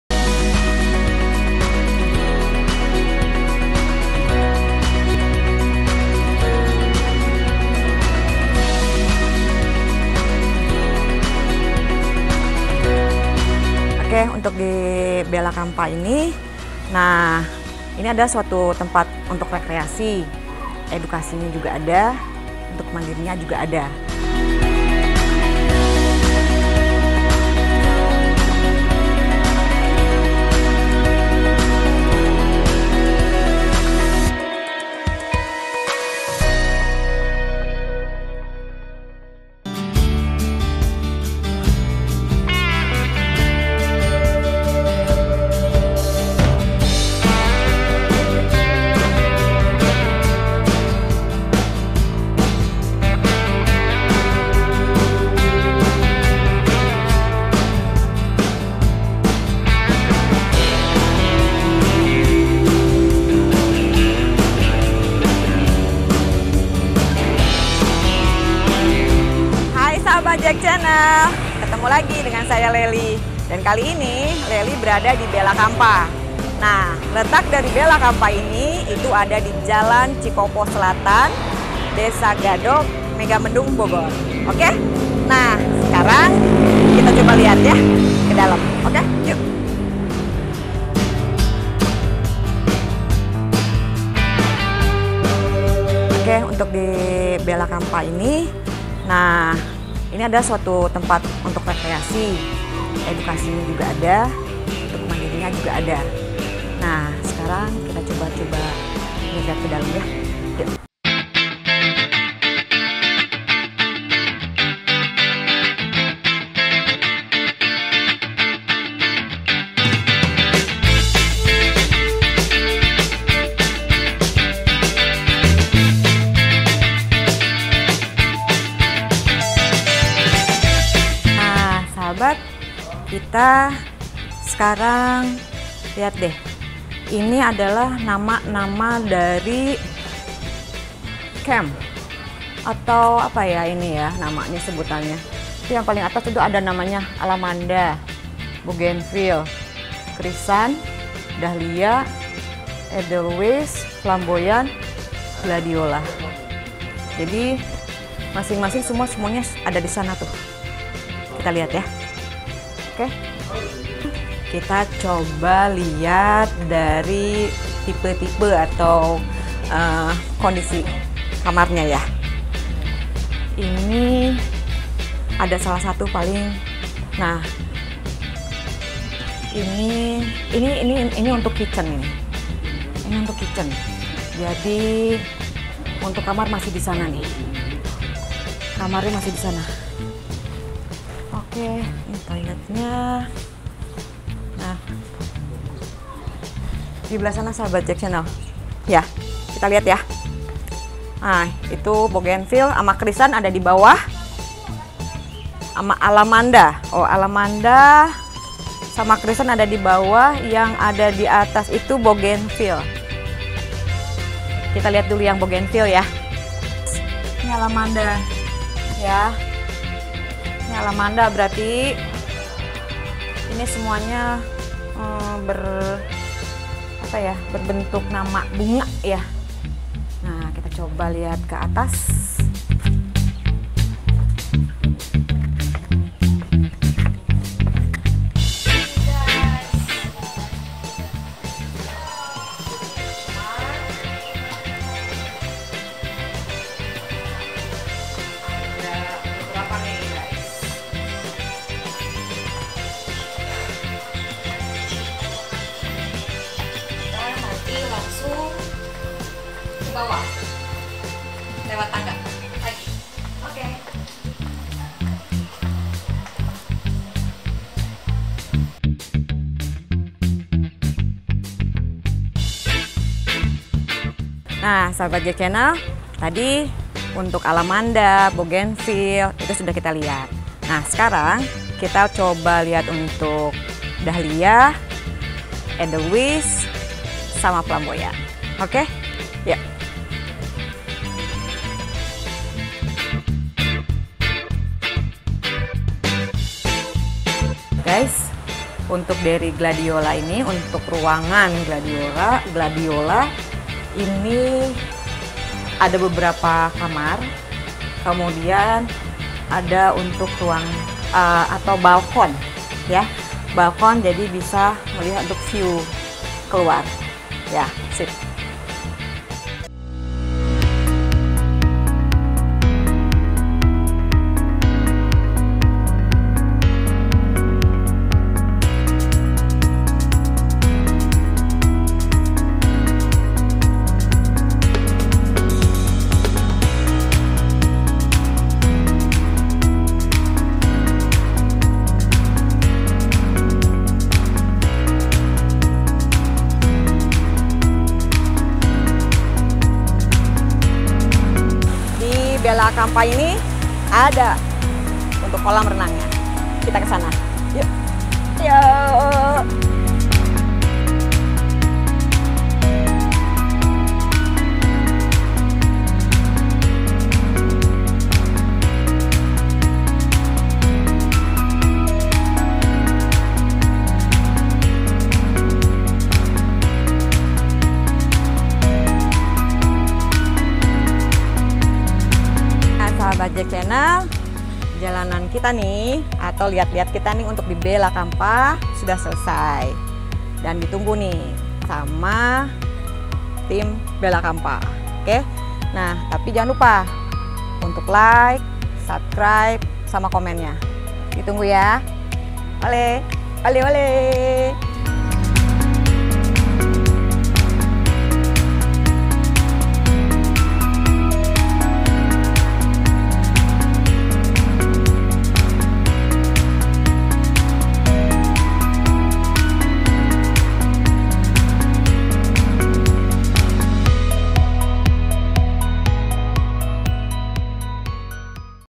BELLA CAMPA HOTEL || Fasilitas Ruang Meeting, Outbond, Rekreasi, Edukasi Dan Mandiri..
Bella Campa terletak di Gadog arah Pasir muncang, tidak jauh dari lampu merah gadog, tempat ini cocok untuk Rekreasi, Edukasi,Outbond, Meeting dan bersantai.
Tempatnya sangat luas, asri dan sejuk, fasilitas, kolam renang, cocok untuk liburan keluarga.